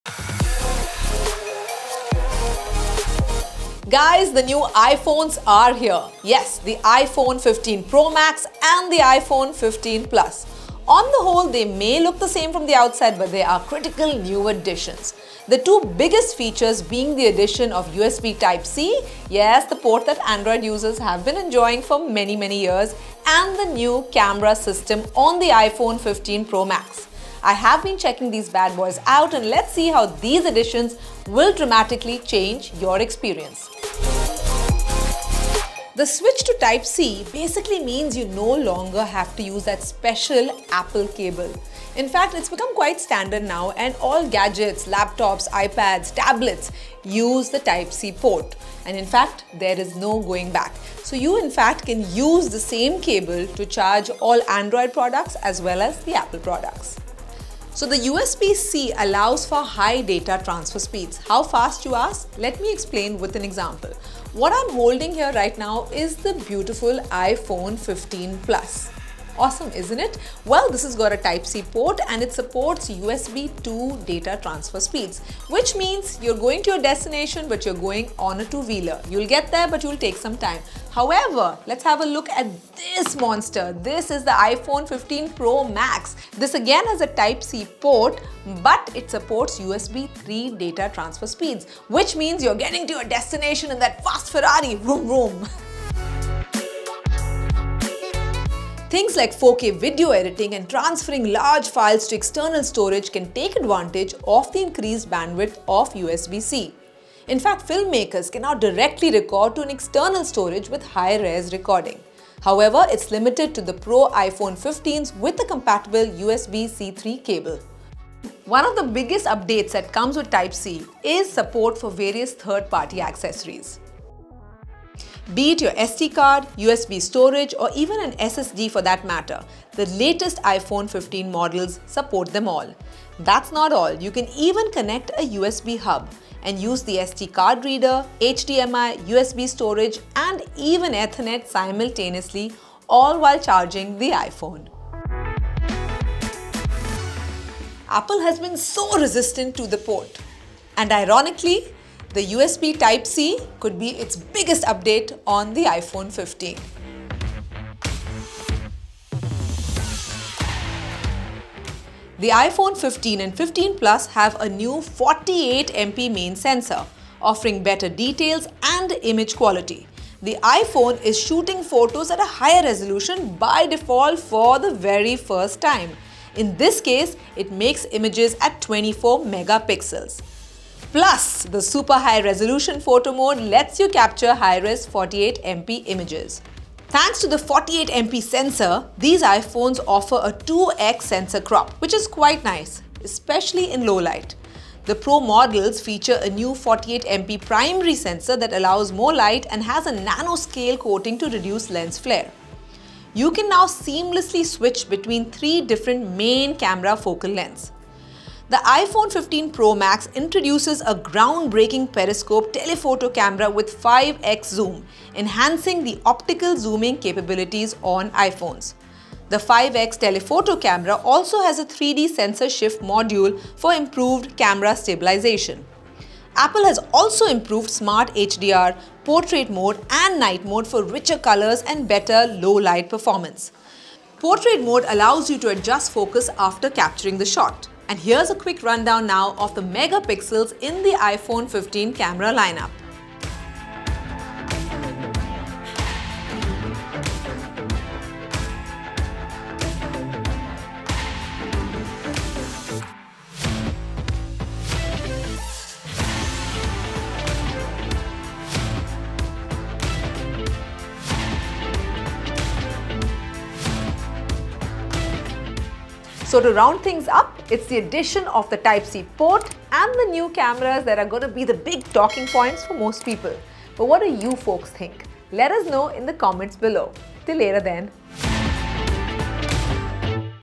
iPhone 15: Why Are Type C Port & Telephoto Cameras a Big Deal?
The iPhone 15 & 15 Pro are the talk of the tech town. In this video, we'll explain two critical aspects that truly make this device stand out: the Type C port and its impressive camera capabilities.